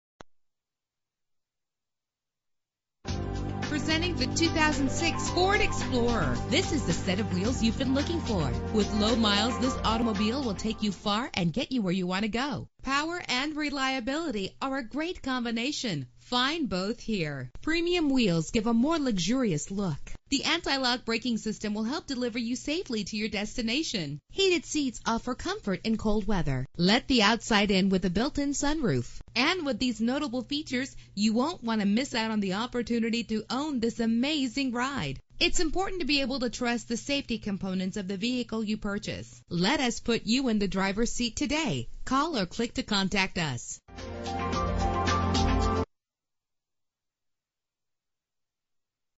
2006 Ford Explorer - Norman OK
Year: 2006
Make: Ford
Model: Explorer
Trim: Eddie Bauer
Engine: 4.0 liter 6 cylinder 12 valve
Transmission: 5-Speed Automatic
Color: Maroon
Mileage: 17665
Address:
821 N.